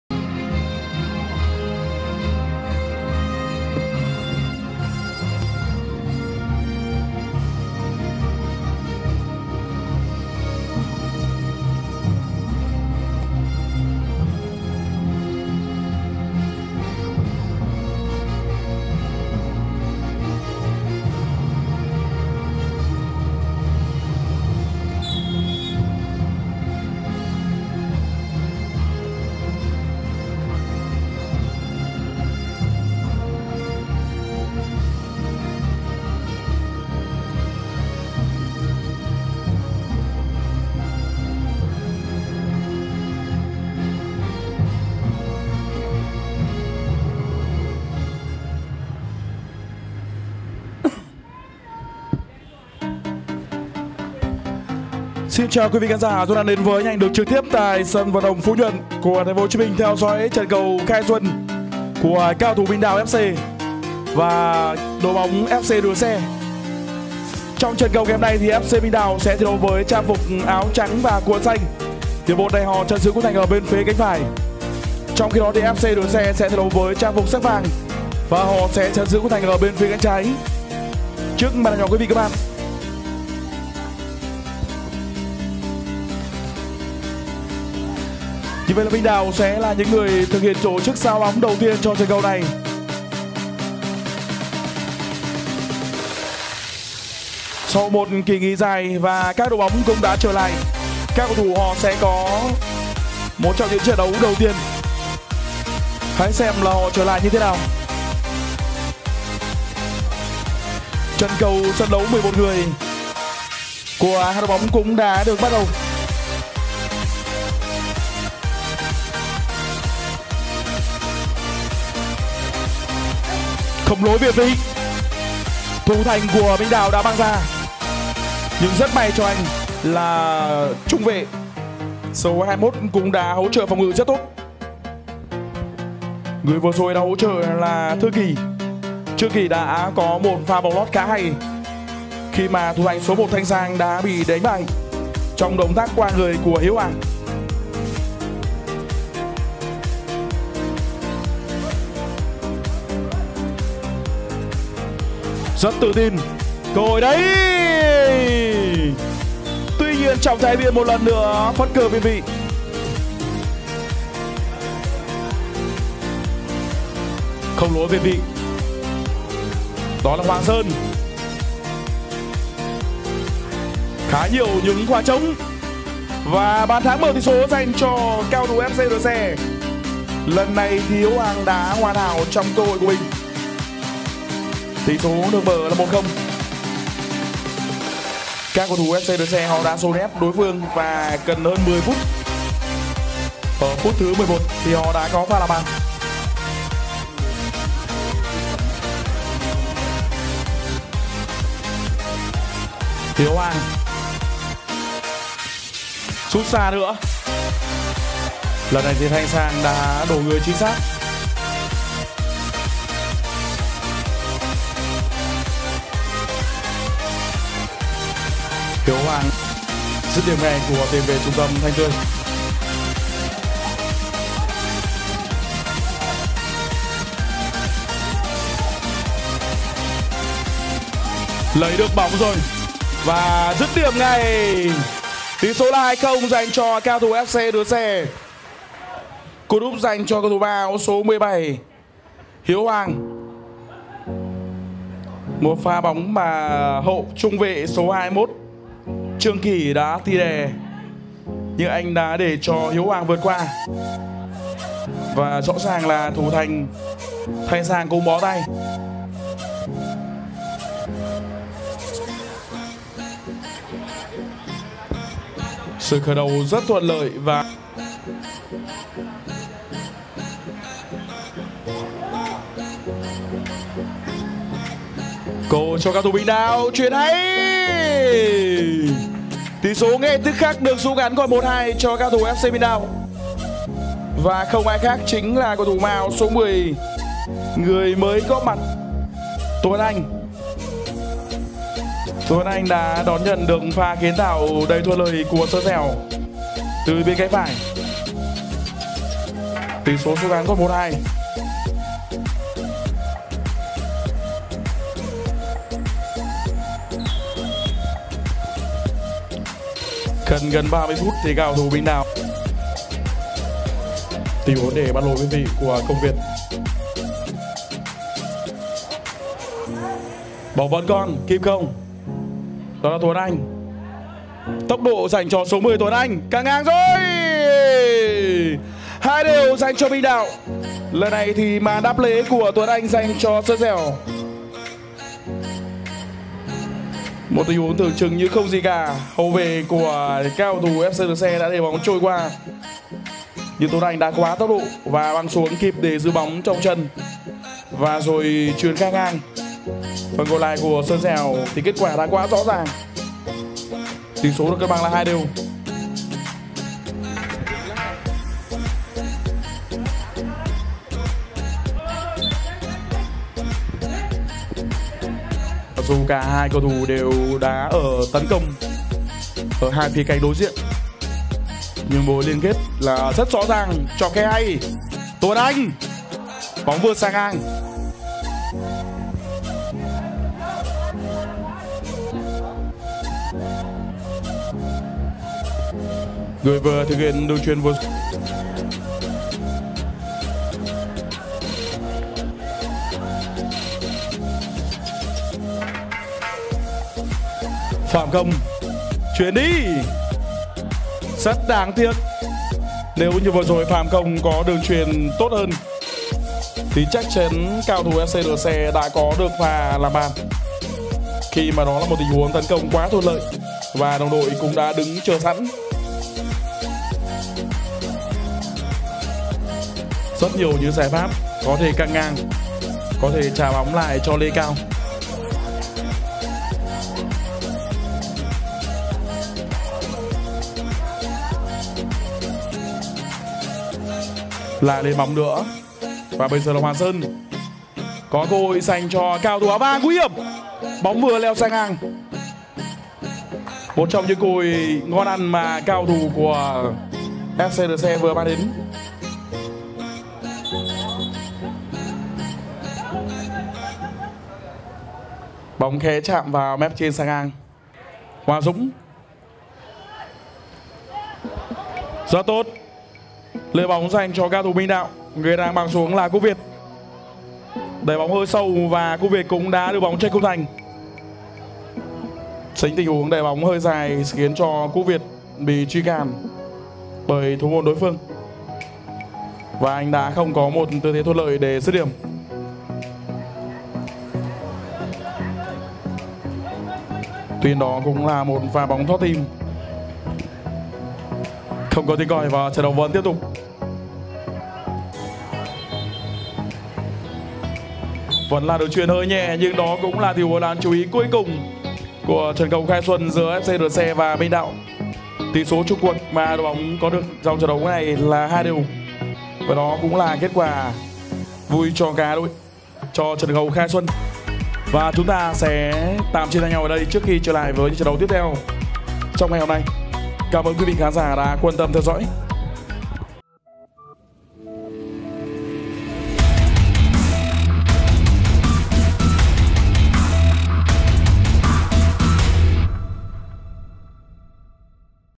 HIGHLIGHT | Minh Đạo FC 2 🆚 2 FC Rửa Xe
HIGHLIGHT | Giao Hữu S11 Khai Xuân Tân Sửu 2021
➡  Minh Đạo FC 2 🆚 2 FC Rửa Xe
GHI BÀN:
- Minh Đạo FC: Cú đúp Hiếu Hoàng (17)
- FC Rửa Xe: Tuấn Anh (10), Xuân Dẻo (40)
⏱ 14:30 Ngày 23/02/2021
🏟 Sân Bóng | Phú Nhuận | Q. Phú Nhuận
🎙 BLV : Anh Nhiên
🎬 Livebongda - Truyền Thông Thể Thao